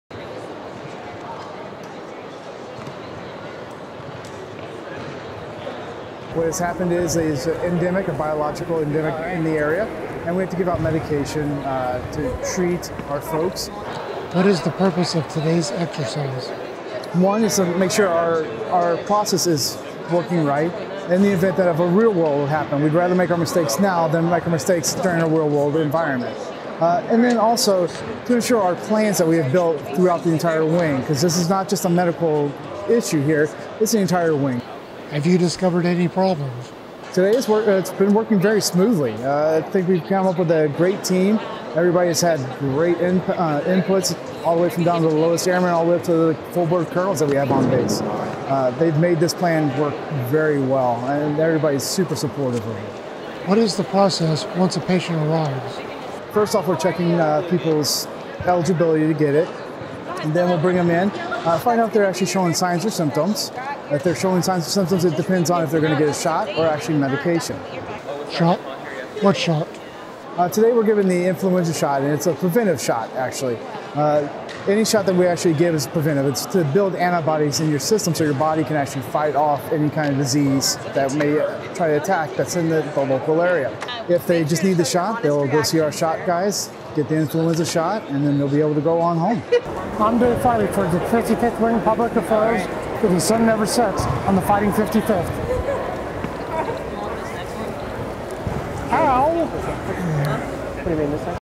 We are ready.
The 55th Wing Medical Readiness Flight recently held an exercise to test their medical readiness.